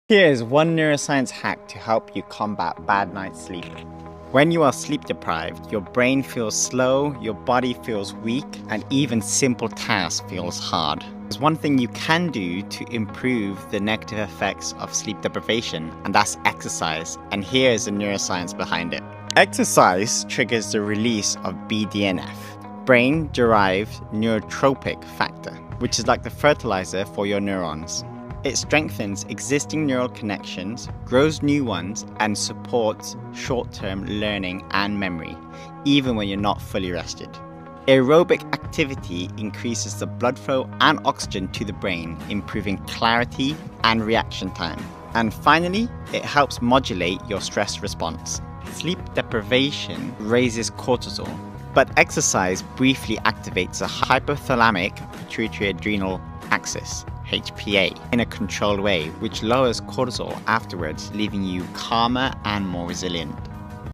How to counteract the negative effects of sleep deprivation
🧠 Sleep deprivation affects far more than energy levels. Missing sleep impairs cognitive performance, slows reaction time, disrupts memory consolidation, and elevates stress hormones like cortisol. It also reduces neuroplasticity, making it harder for the brain to learn and adapt.

Research shows that moderate-intensity exercise can help counteract some of these effects. Around 20 to 30 minutes of continuous movement such as brisk walking, cycling, or light jogging can significantly improve alertness, working memory, and decision-making after a poor night’s sleep.

Exercise increases blood flow to the brain, delivers oxygen and nutrients for mental clarity, boosts dopamine and norepinephrine for focus and mood, and elevates brain-derived neurotrophic factor (BDNF), which supports neuron growth and repair. It also helps reset your circadian rhythm, making it easier to achieve deeper, restorative sleep the next night.

While it cannot replace the full benefits of quality sleep, short bouts of movement can give your brain a measurable performance boost when you are running on limited rest.